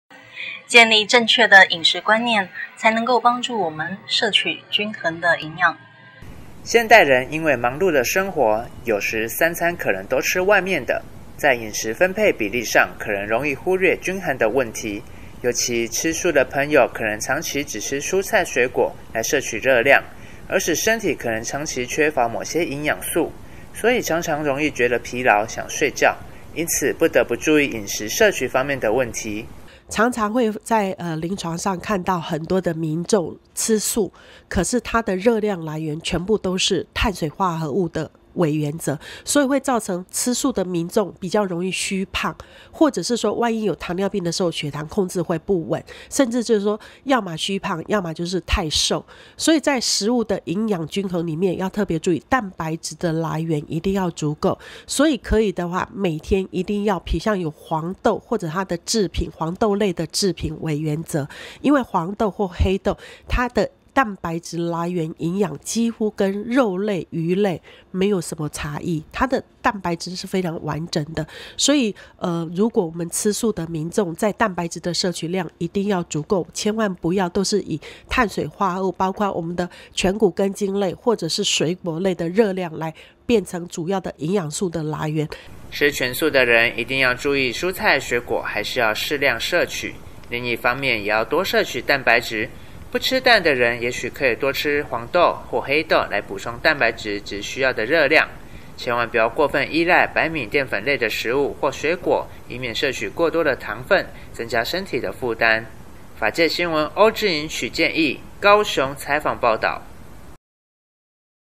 法界新聞》20151230飲食觀念要正確，均衡攝取不過量
法界新聞》
受訪者：高雄市立聯合醫院 營養科  謝主任
現代人因為忙碌的生活，有時三餐可能都吃外面的，在飲食分配比例上可能容易忽略均衡的問題，尤其吃素的朋友可能長期只吃蔬菜水果來攝取熱量，而使身體可能長期缺乏某些營養素，所以常常容易覺得疲勞，想睡覺，因此不得不注意飲食攝取方面的問題，